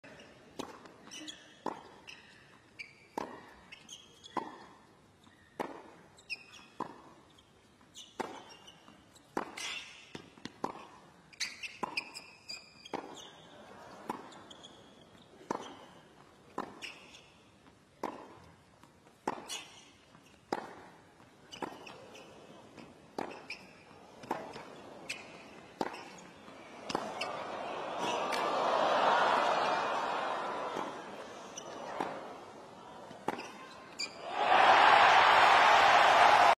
Jannik Sinner's UNBELIEVABLE forehand winner! 🔥
Jannik Sinner won this amazing point against Daniil Medvedev in the final of the Australian Open 2024.